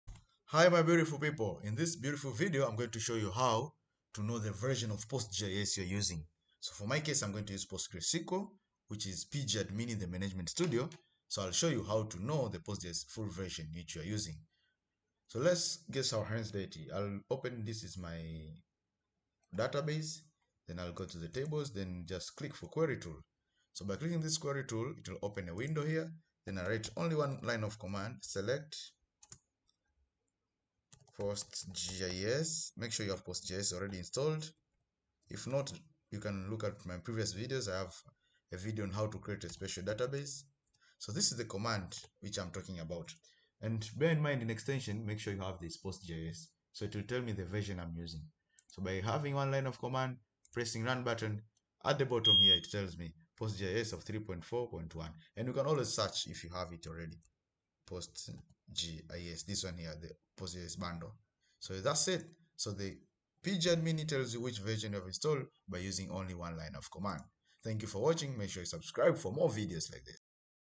How to know PostGIS version using PgAdmin in PostgreSQL
The video shows how to know the PostGIS version using PgAdmin using PostgreSQL.